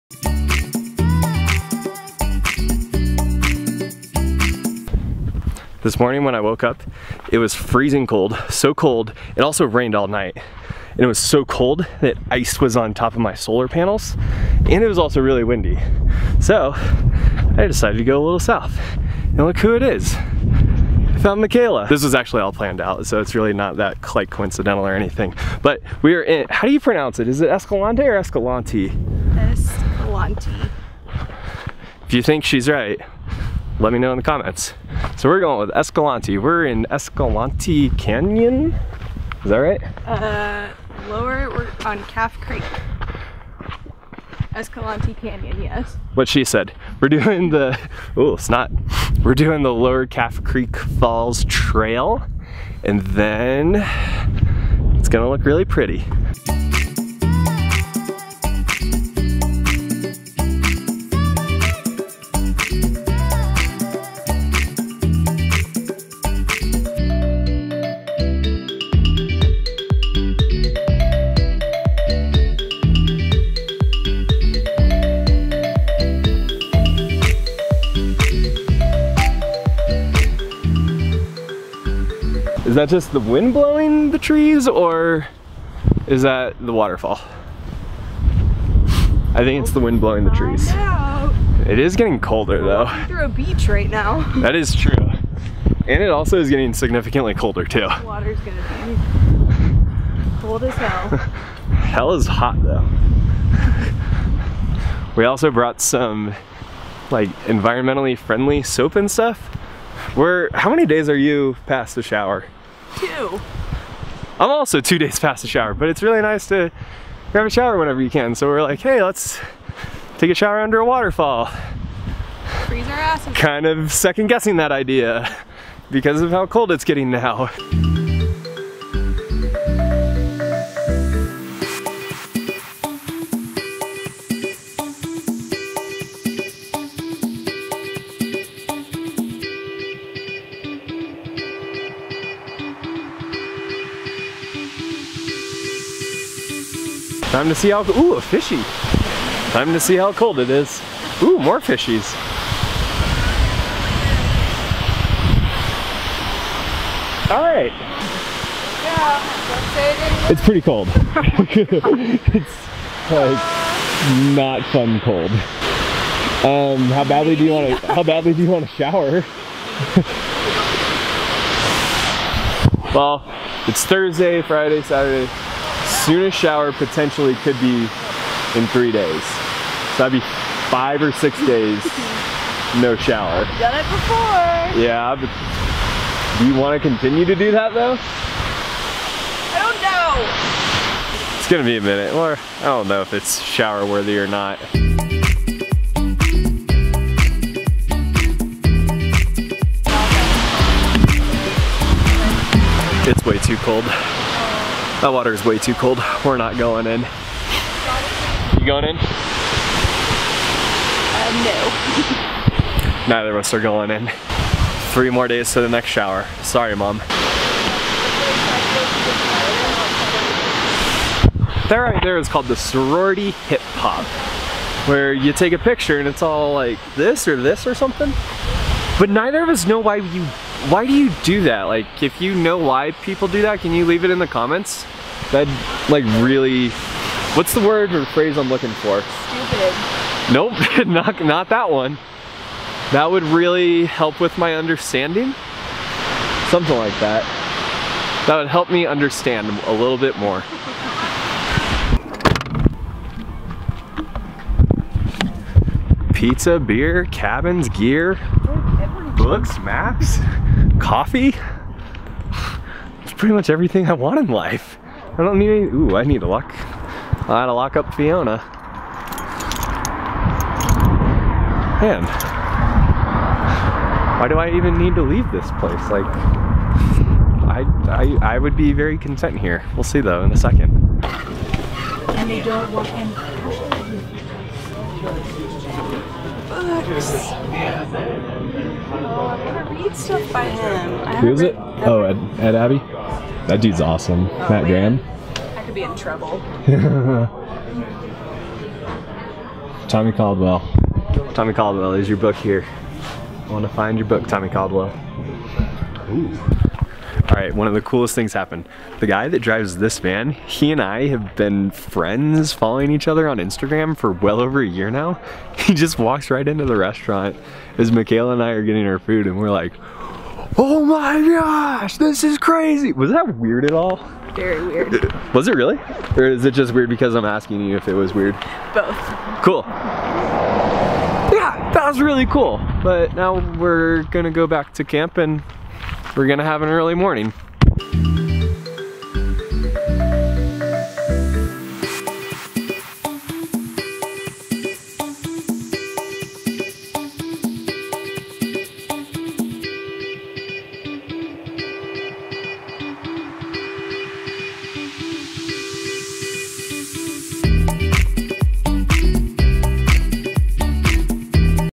Lower Calf Creek Falls Hike
Daily Vlog Episode 093
VanLife Day 39

It got cold, real cold, at Joe's Valley. I headed south to Grand Staircase-Escalanete National Monument to meet up with Mikaela for a hike at Lower Calf Creek Falls.

Video Gear:
Big Camera
Little Camera
Bendy Tripod
Big Tripod

Have a question? Drop it in the comment section.

If you got this far. You're rad. Up top! *high five*